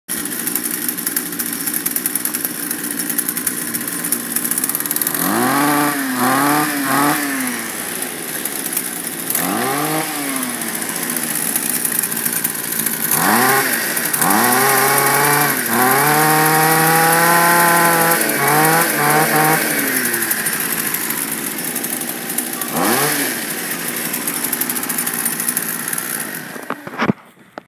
Shindaiwa T222 Home Pro weed trimmers | For Sale | Online Auction
Lot of (2) Shindaiwa T222 Home Pro weed eater/weed whips/spin trimmers, both run but will need some work